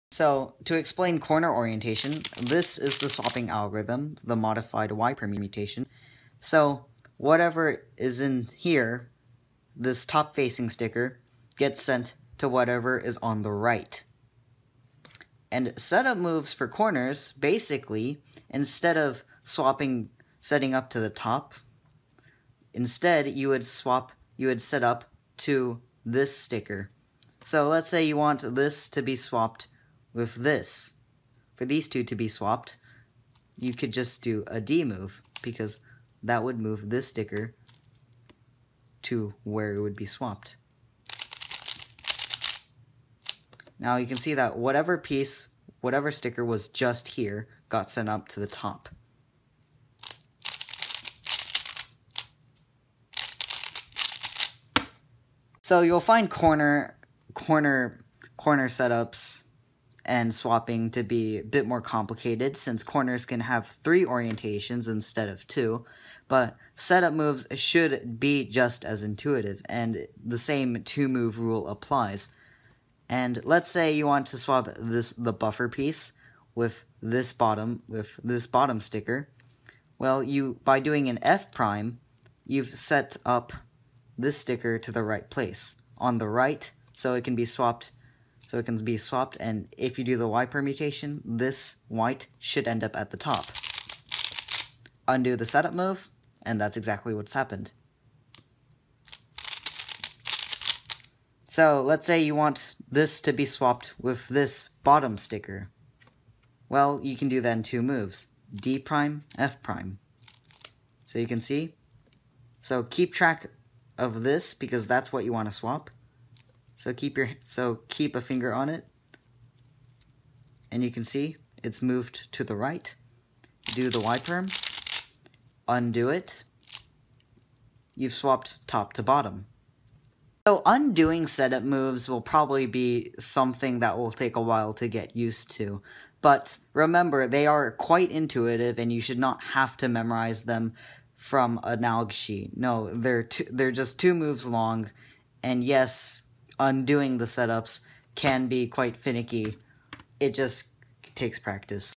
How to Solve a Rubik's Cube Blindfolded - Part 3: Corners: Setup Moves and Orientation
I forgot to have an example cycle for corners... If you want one, you'll find one in part 4 with the full scramble example solve.